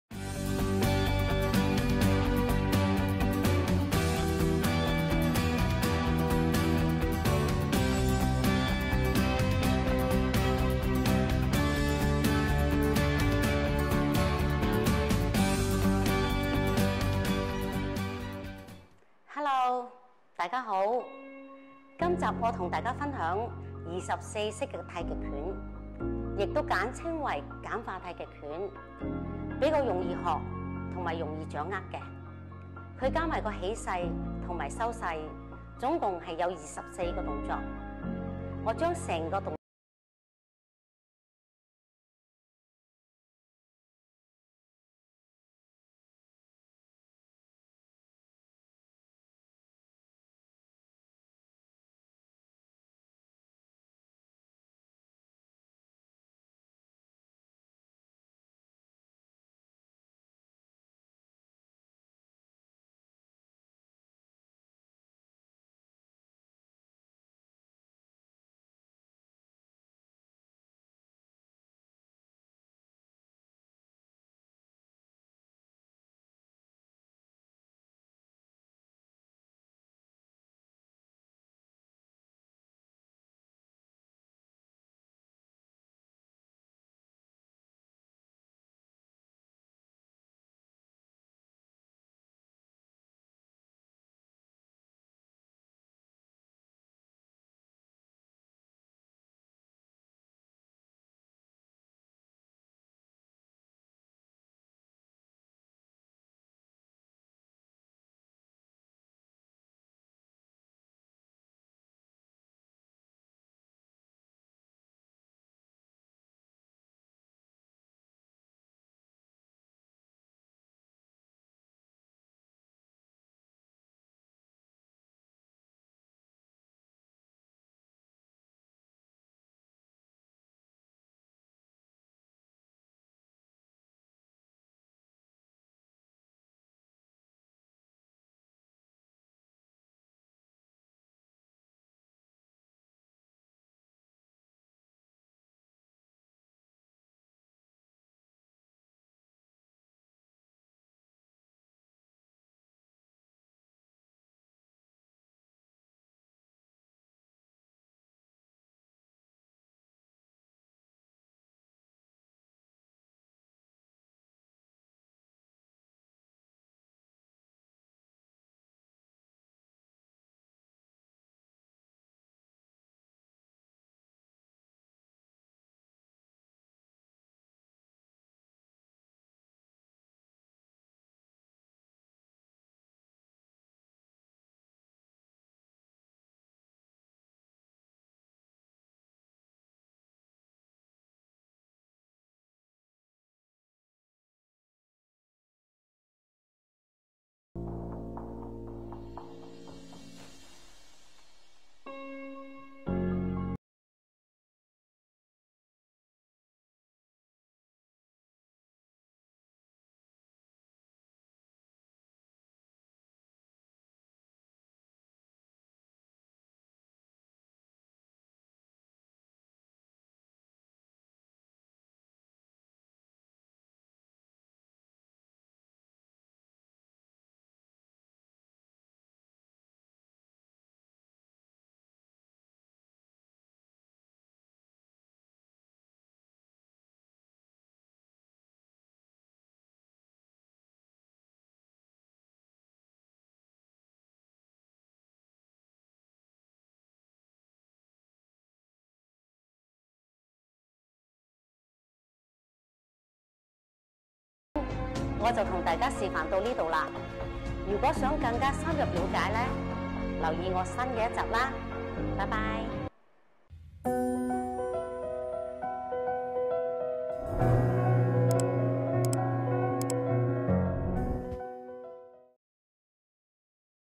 Yeung Ching Ching｜24 Form Tai Chi Quan - Full Demonstration｜ 楊菁菁｜二十四式太極拳 - 全套示範
太極拳是一項鬆柔自然的運動,二十四式太極拳稱(簡化太極拳),是按照由簡到繁,由易到難的原則,易學易懂,這套太極拳包括(起勢)、(收勢)共二十四個姿勢動作。

Tai Chi Quan is a soft and natural movement. Twenty-four styles of Taijiquan are called (simplified Tai Chi quan). It is easy to learn and understand according to the principle of from simple to complex and from easy to difficult.  A total of 24 postures and actions.